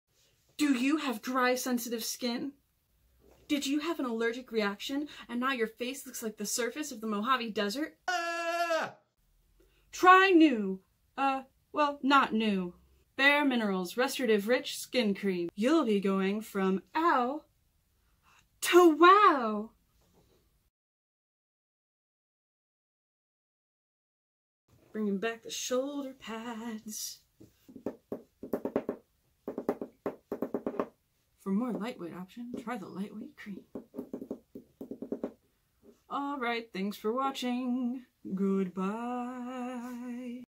I made a BAREMINERALS commercial for some reason
*special appearance by Ed's tweed jacket*

I basically made this because I had 2 allergic reactions on my face in a close span of time and it sucked - a lot. The only thing I could put on that helped was this moisturizer so I love it now. 👰